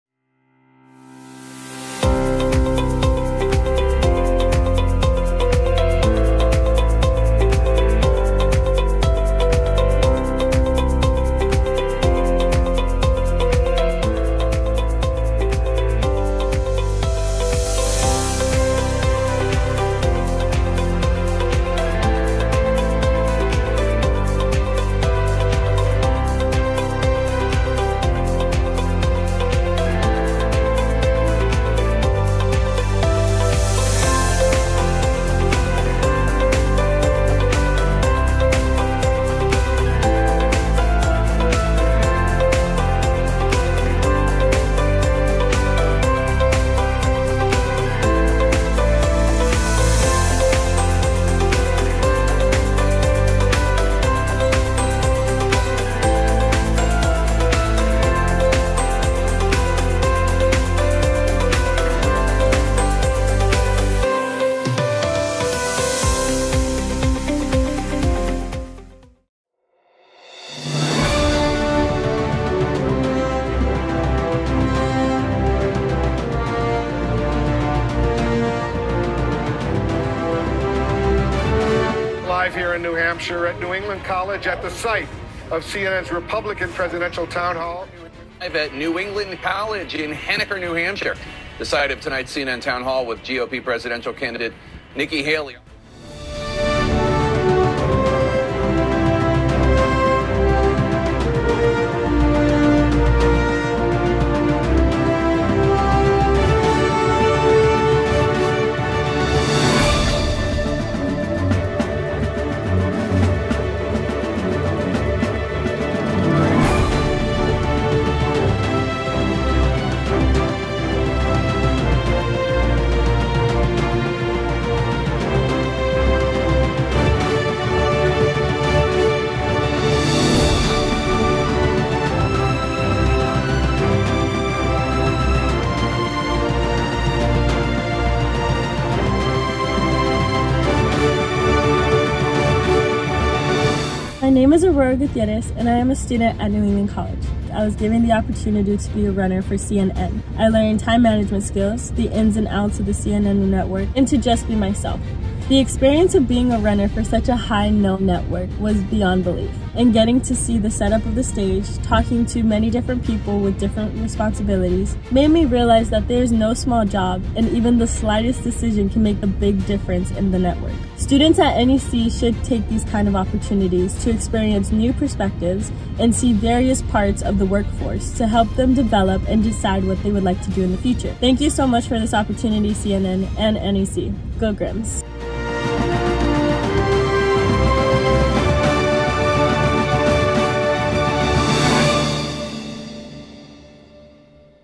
CNN at New England College | Republican Presidential Town Hall Meetings
Welcoming CNN to campus was an incredible experience for the entire community. Many of our faculty, students, and staff took part in helping CNN Politics successfully host two Presidential Town Hall meetings with Wolf Blitzer, Jake Tapper, Ron DeSantis, and Nikki Haley.

Watch our video recap and hear directly from one of our hard-working students who was a runner throughout the process! 🙌🏽🎥